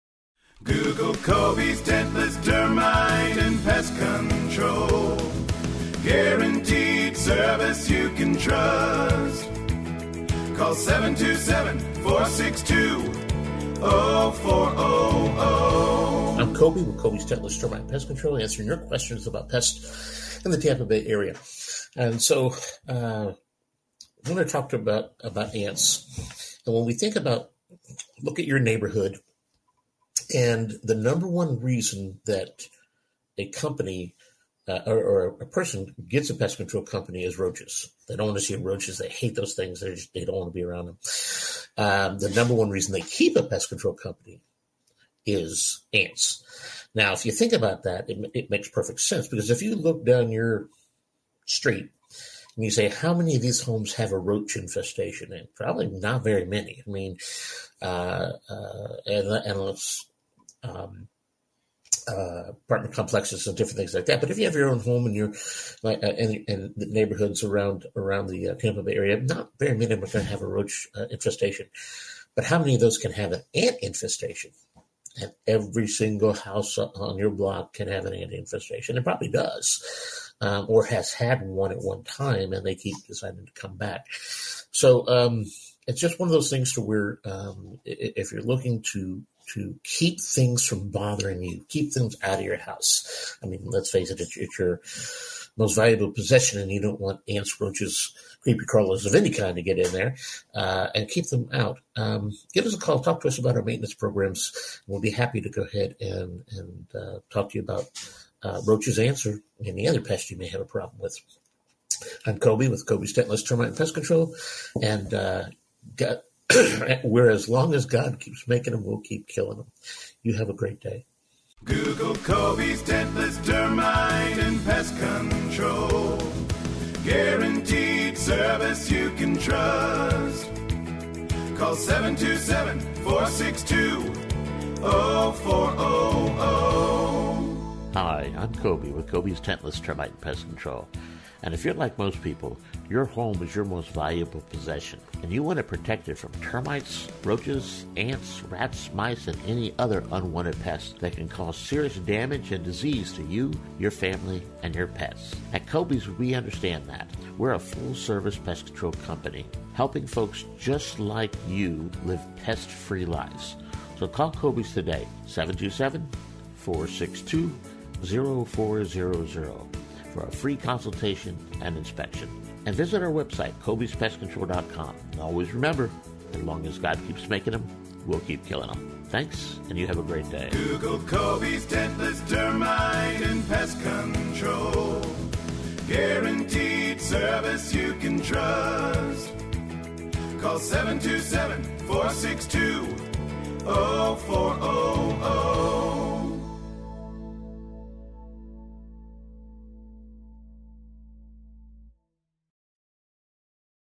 Number 1 Reason To Keep Your Pest Control Company
And we’re back. I’m Coby, Coby tentless termite pest control answering your questions about pest in the Tampa Bay area. And so we’re gonna talk about about ants. And when we think about look at your neighborhood. And the number one reason that a company or a person gets a pest control company is roaches, they don’t assume roaches, they hate those things. They don’t want to be around them. The number one reason they keep a pest control company is ants. Now, if you think about that, it makes perfect sense. Because if you look down your street, and you say how many of these homes have a roach infestation and probably not very many, I mean

apartment complexes of different things like that. But if you have your own home, and you’re in the neighborhoods around around the Tampa Bay area, not being able to have a roach infestation. But how many of those can have an ant infestation? Every single house on your block can have an infestation, it probably does, or has had one at one time and they keep deciding to come back. So it’s just one of those things to where if you’re looking to to keep things from bothering you keep them out of your house. I mean, let’s face it, it’s your most valuable possession and you don’t want ants, roaches, maybe crawlers of any kind to get in there and keep them out. Give us a call. Talk to us about our maintenance programs. We’ll be happy to go ahead and talk to you about roaches answer any other pests. You may have a problem with Coby with Coby Tentless Termite and Pest Control and get where as long as God keeps making them. We’ll keep killing them. You have a great day
The post Number 1 Reason To Keep Your Pest Control Company first appeared on Coby's Pest Control